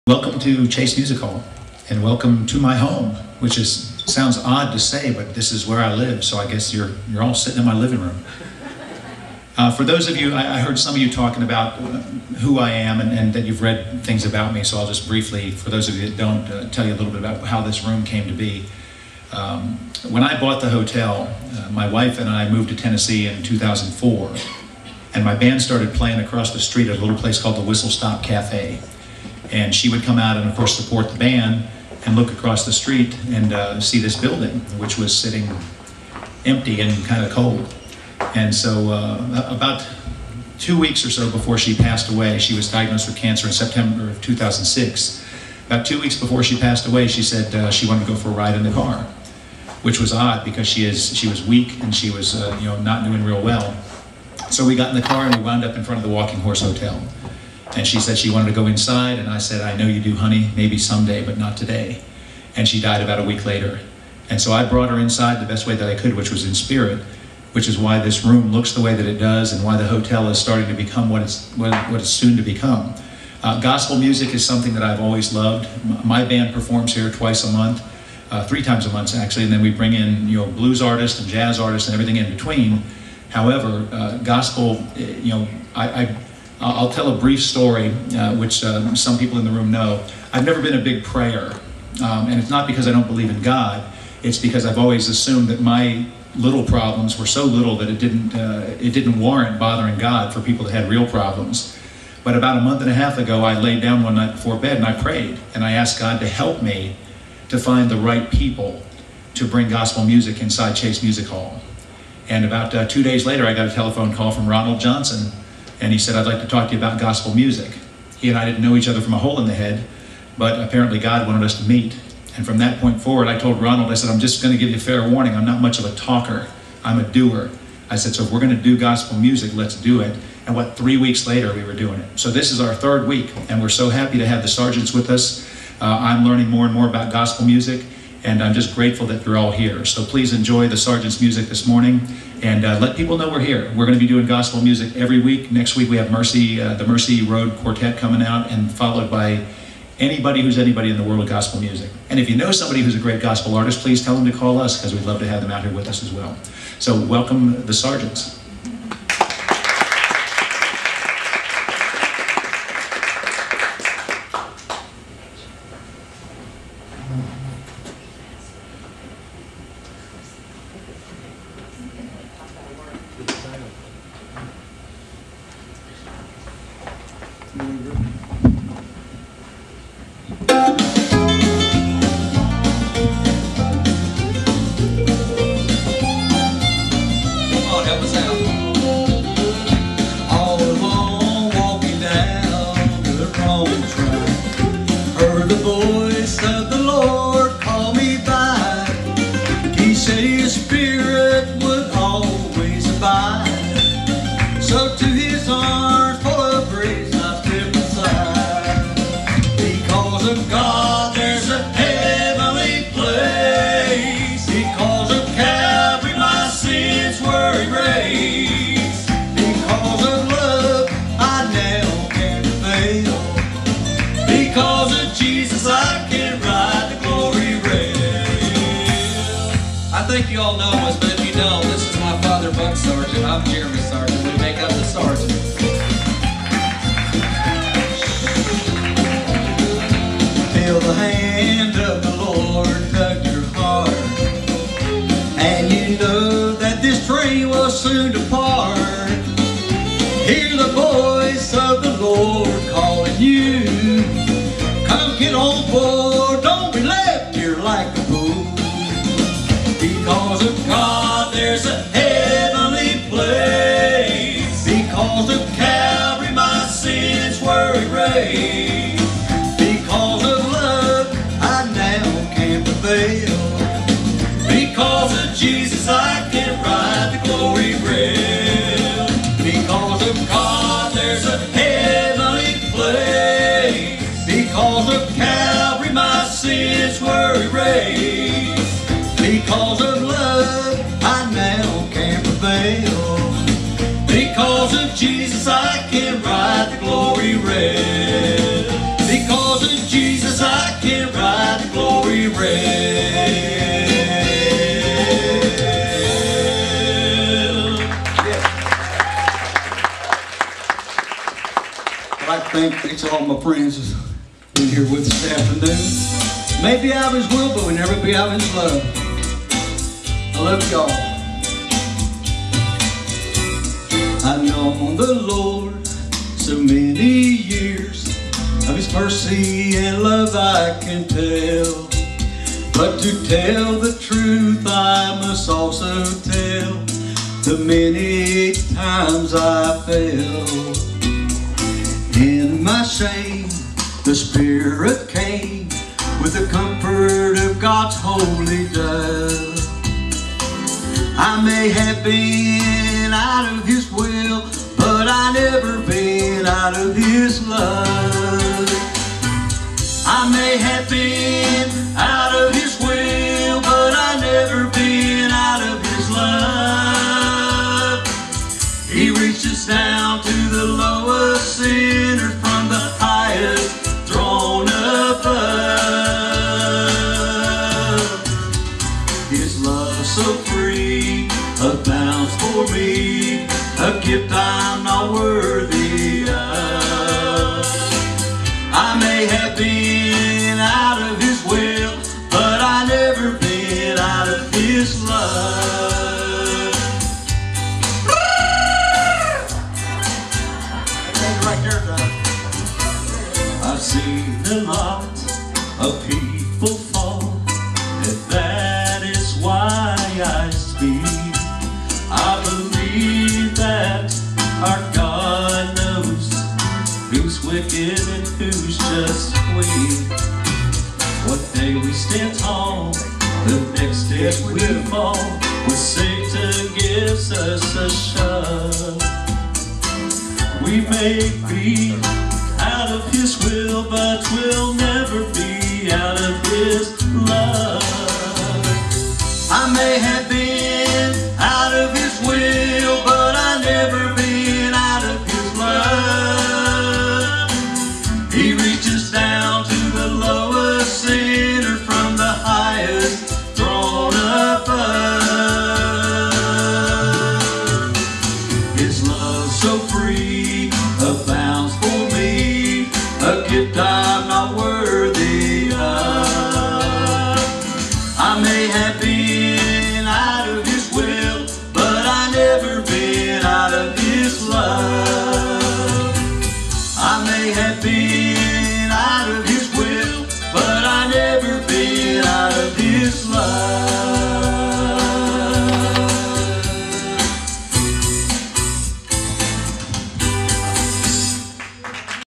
THE SARGENTS APRIL 25, 2010
The Sargents at Chais Music Hall / Walking Horse Hotel.  April 25, 2010.  Great group and concert.  Go see them when their in your area, you will be glad you did.  God Bless.